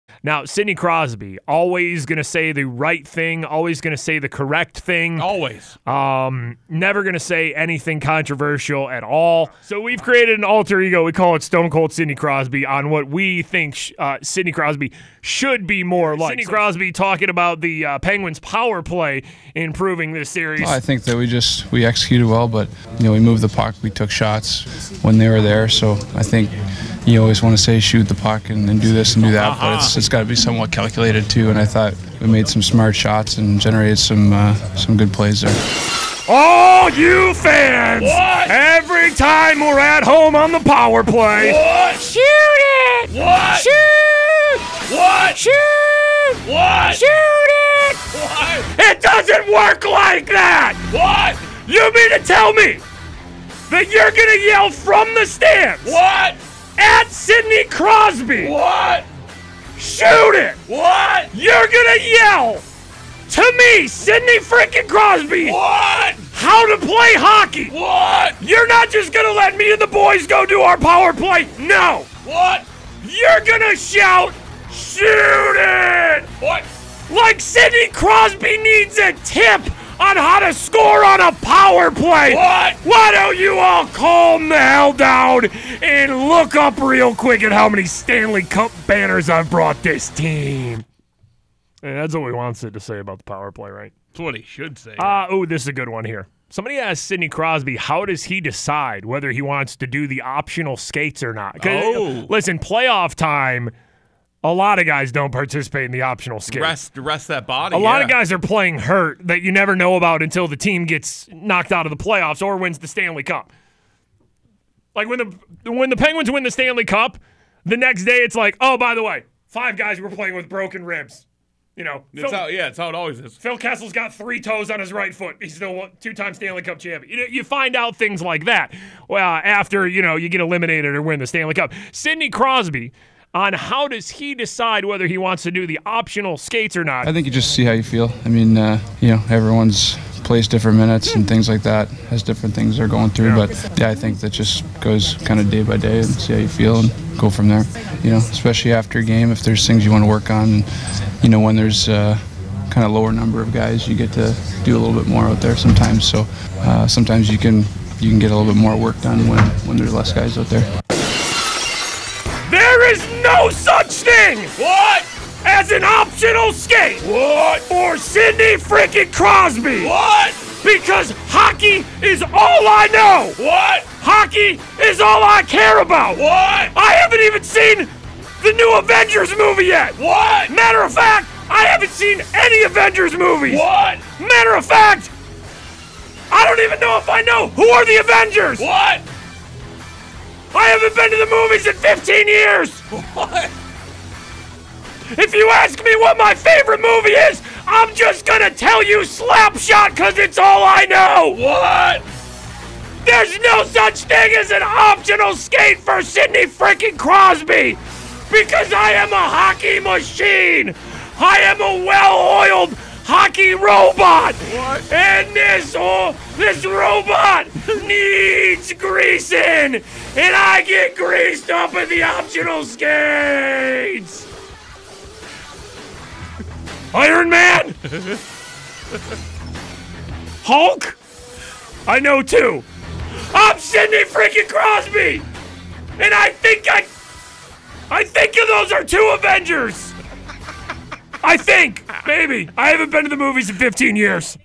Stone Cold Sidney Crosby on power play and optional skate
This is how we wish Sidney Crosby would respond to questions about the power play and optional skates